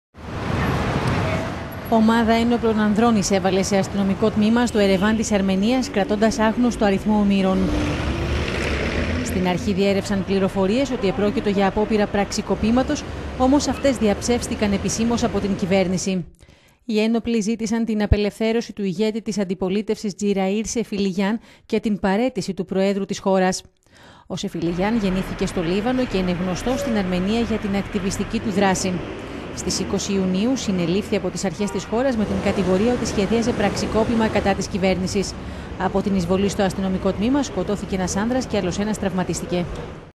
Αρμενία: Ένοπλοι εισέβαλαν σε αστυνομικό τμήμα και κράτησαν ομήρους
Ομάδα ένοπλων ανδρών εισέβαλε σε αστυνομικό τμήμα στο Ερεβάν της Αρμενίας κρατώντας άγνωστο αριθμό ομήρων.

Στην αρχή διέρρευσαν πληροφορίες ότι επρόκειτο για απόπειρα πραξικοπήματος, όμως αυτές διαψεύστηκαν επισήμως από την κυβέρνηση.

Οι ένοπλοι ζήτησαν την απελευθέρωση του ηγέτη της αντιπολίτευσης Τζιραίρ Σεφιλιγιάν και την παραίτηση του προέδρου της χώρας.

 Ο Σεφιλιγιάν γεννήθηκε στο Λίβανο και είναι γνωστός στην Αρμενία για την ακτιβιστική του δράση. Στις 20 Ιουνίου συνελήφθη απο τις α…